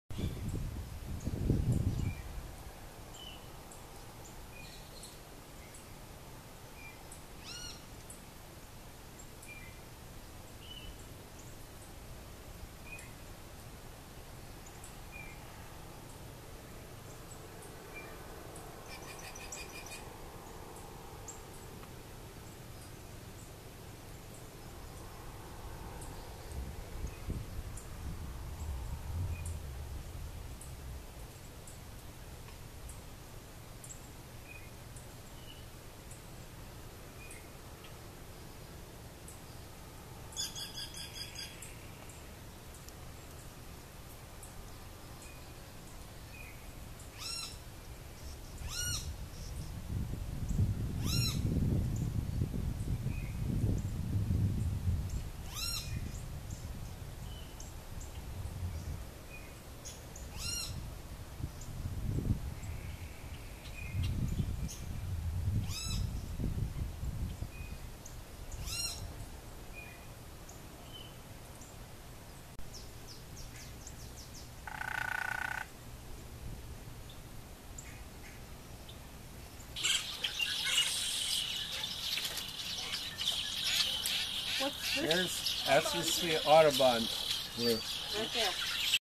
Altamira Oriole Feeding on an Orange Resaca De La Palma State Park and Salineno
Altamira Oriole Feeding on an Orange
Resaca De La Palma State Park
Cameron County Texas February 9, 2015
Last Clip Salineno Starr County Texas February 10, 2015